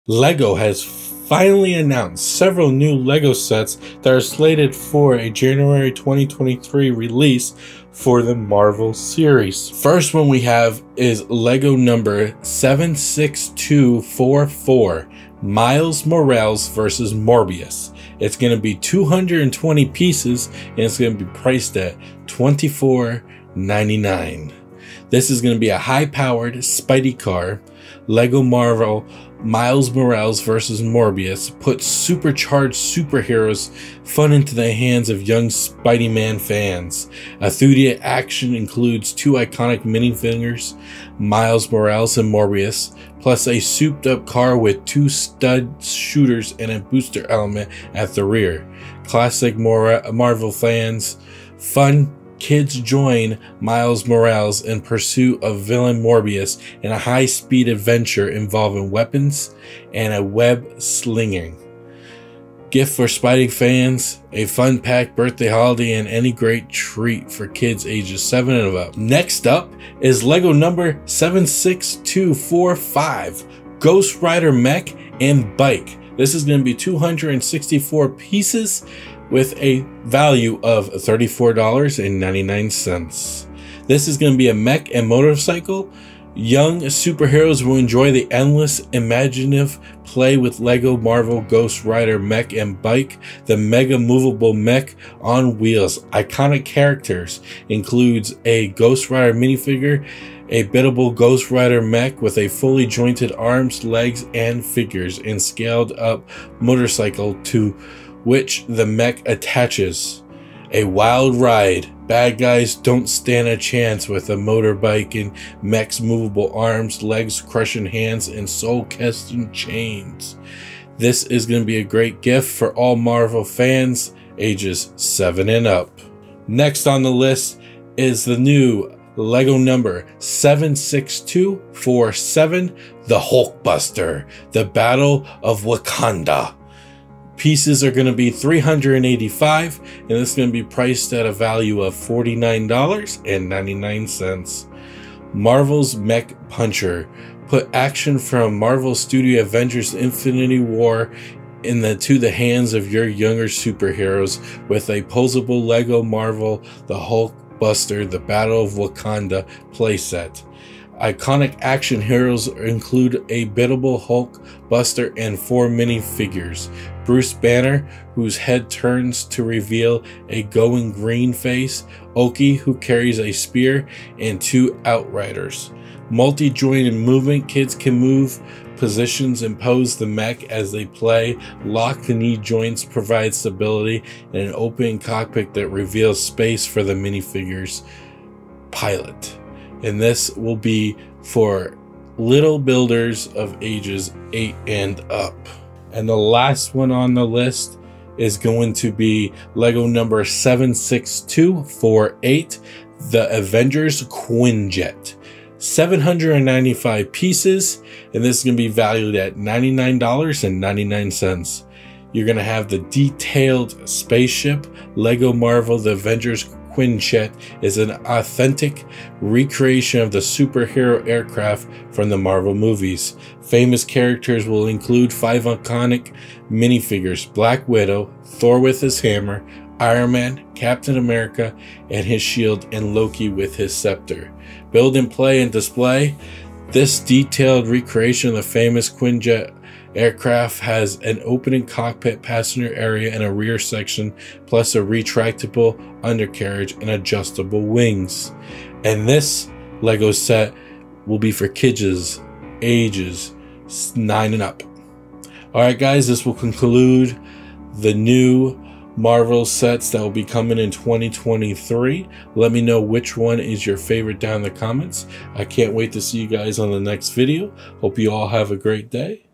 LEGO Marvel January 2023 OFFICIAL Set Reveal
LEGO has announced several new LEGO sets that are slated for a January 2023 release.
76244 Miles Morales vs Morbius - 220 pieces - $24.99
76245 Ghost Rider Mech & Bike - 264 pieces - $34.99
76247 The Hulkbuster: The Battle of Wakanda - 385 pieces - $49.99
76248 The Avengers Quinjet - 795 pieces - $99.99

- Follow Links Below -